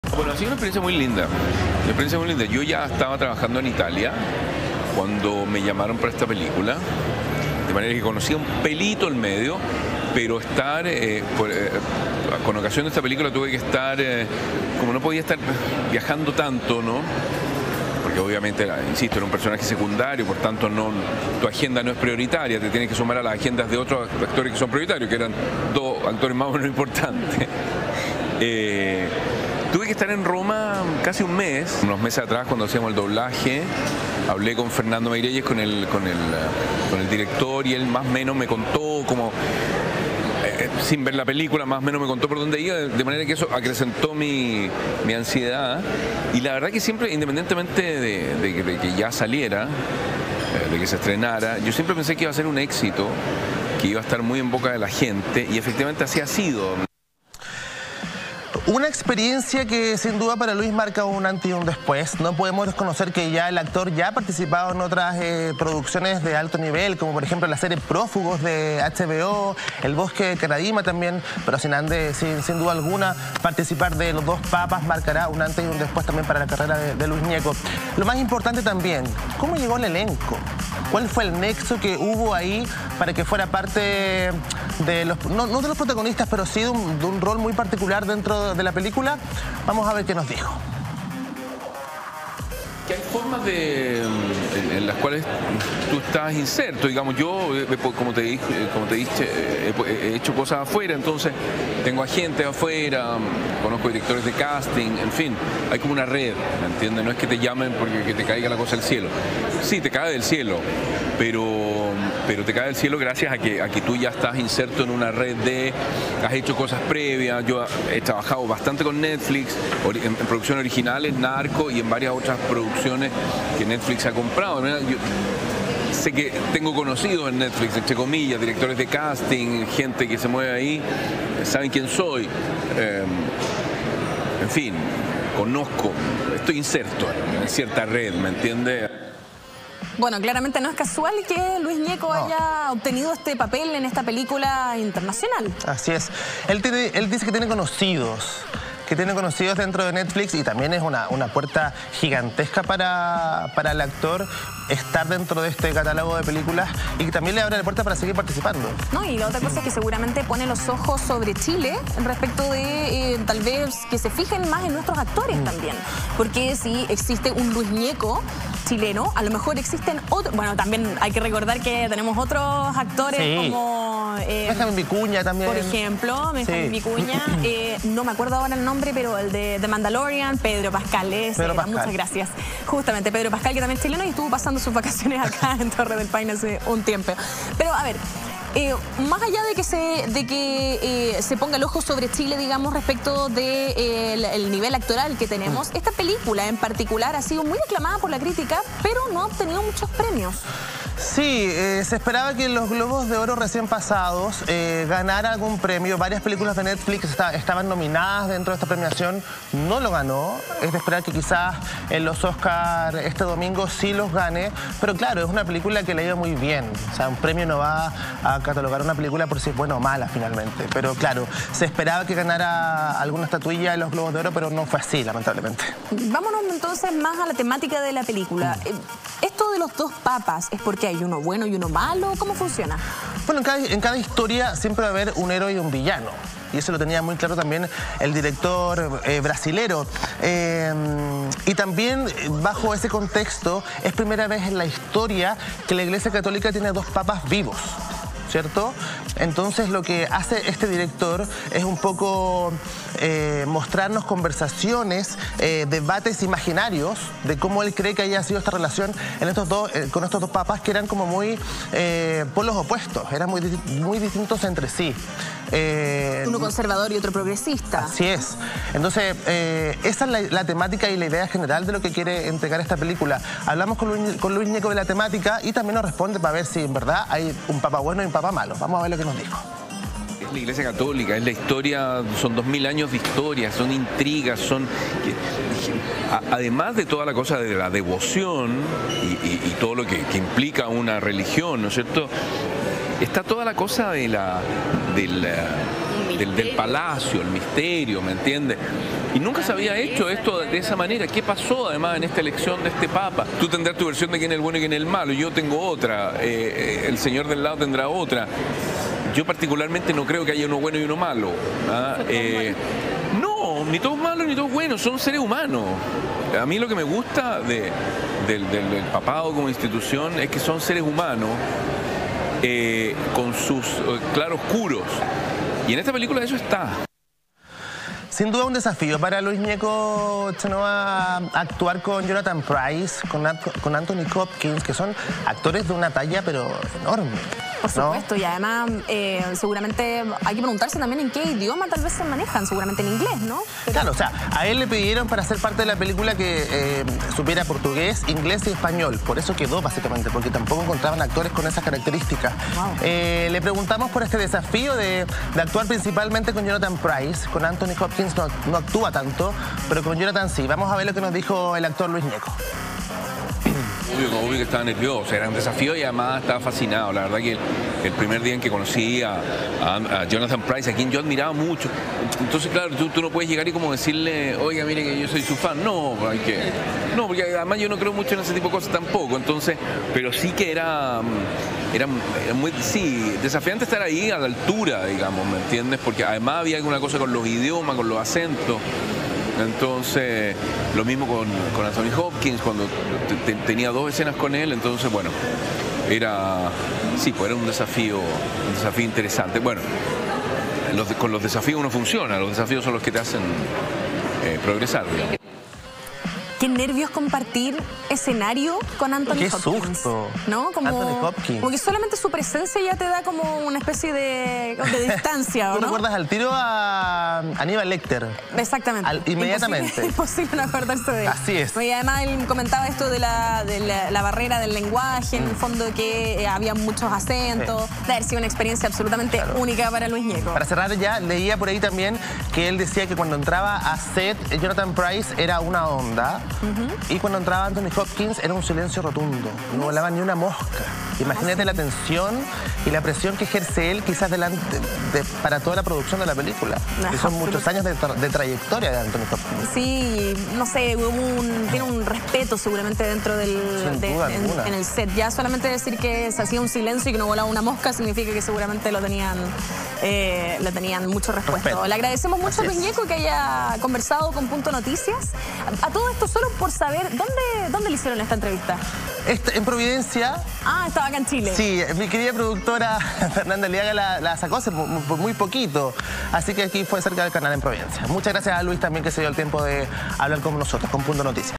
Luis Gnecco nos contó sobre su rol en el elenco de "Los Dos Papas"
Pablo Candia entrevistó a este destacado chileno, quien le contó sobre su papel en este nuevo filme. Entérate de todo en Punto Noticias.